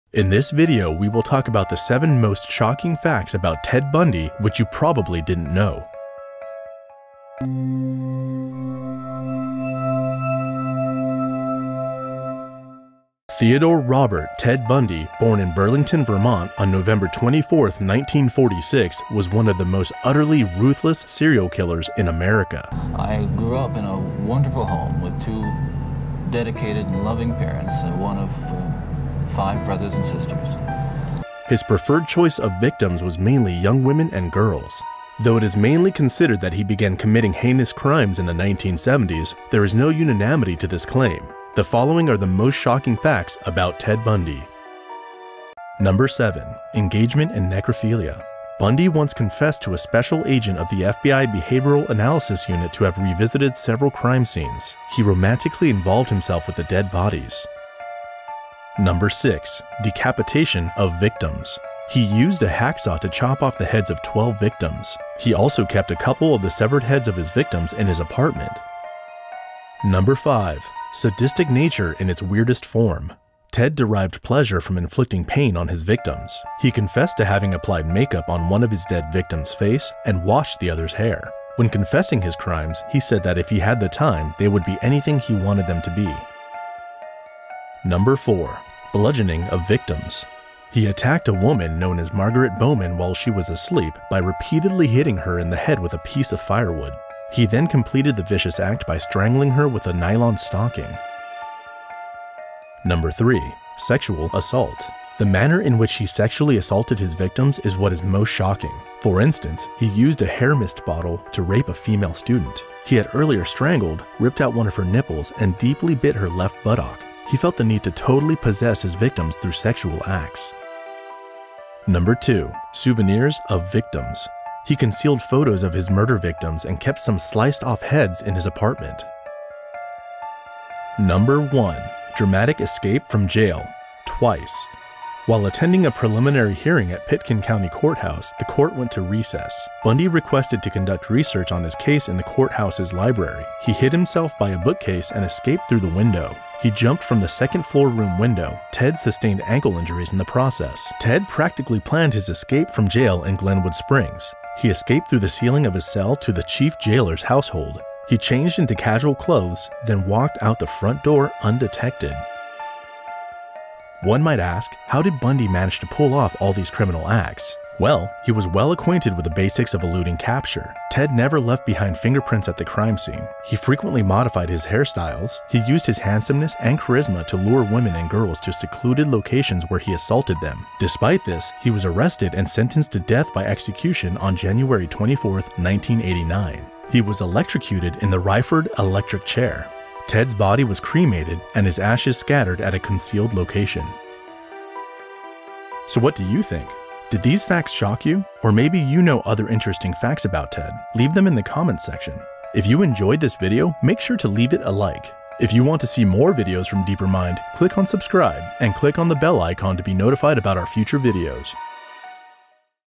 7 shocking facts about Ted Bundy
Ted Bundy was one of the most utterly ruthless serial killers in America. His preferred choice of victims was mainly young women and girls.